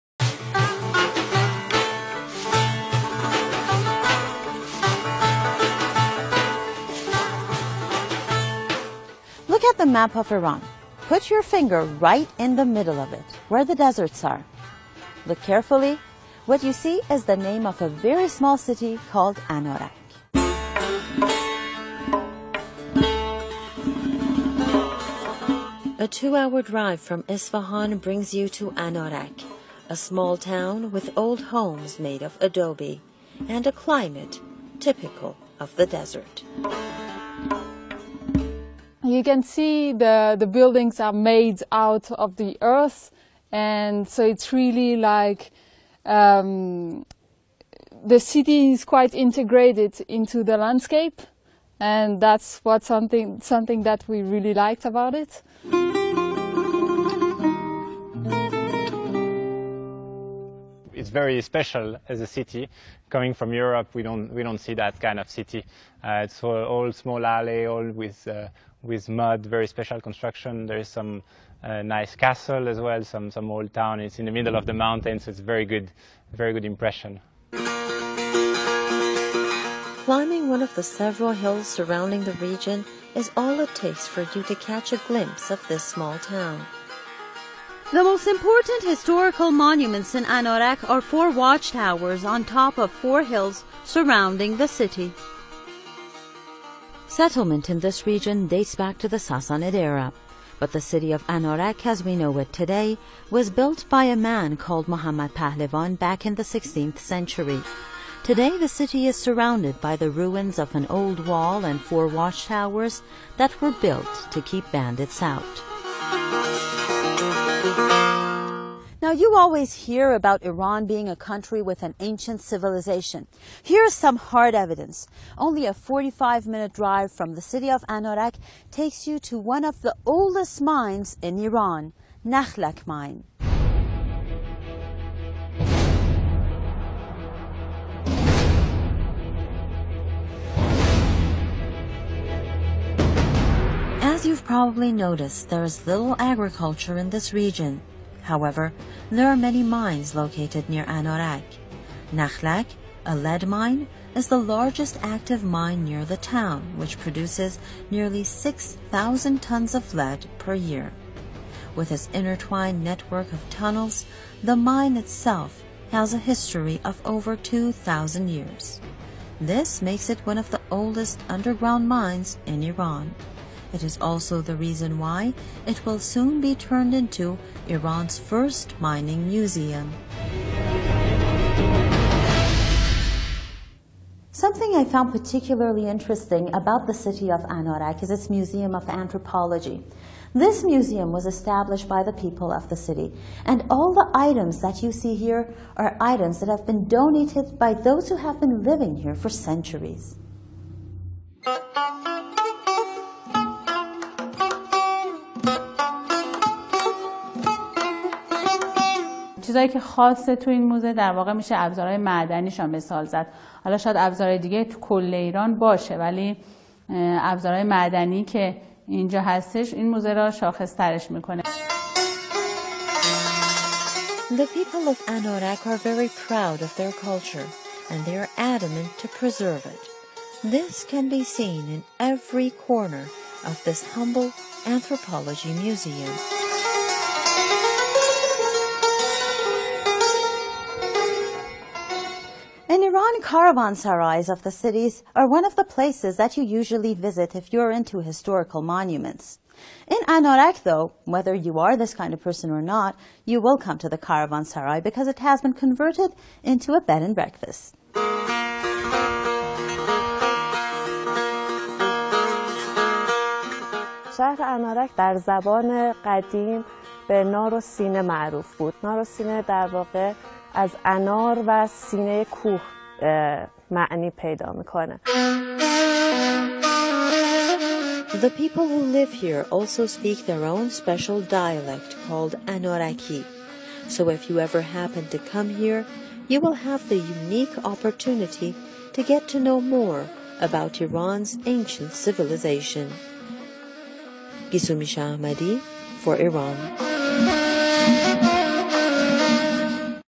Anārak is a city in Isfahan Province, Iran
Anarak (Persian: انارک, also Romanized as Anārak) is a city and capital of Anarak District, in Nain County, Isfahan Province, Iran. At the 2006 census, its population was 1,285, in 462 families. It is situated at an altitude of 1,429 metres (4,688 ft).
Anarak is located at the edge of the "Dasht-e Kavir" and about 75 kilometres (47 mi) from the city of Nain. The city is surrounded by ruins of an old wall and three watchtowers, which were built about 100 years ago to keep Hossein Kashi and his bandit gang out. There is little agriculture in this region, but there are many mines located near Anarak. Nakhlalk, a lead mine, is the largest active mine near Anarak.
The people in Anarak speak a dialect called Anaraki. This language is spoken in Anarak and cities within 65 kilometres (40 mi) of it, such as Chopanan. There is also a museum in Anarak about its history; you may also purchase books with the family tree of families from Anarak.
Anarak's climate is a typical desert climate, little rainfall, hot dry summer days and cool nights. Most of the old homes are adobe and have a courtyard; people move from south to north of the courtyard from winter to summer.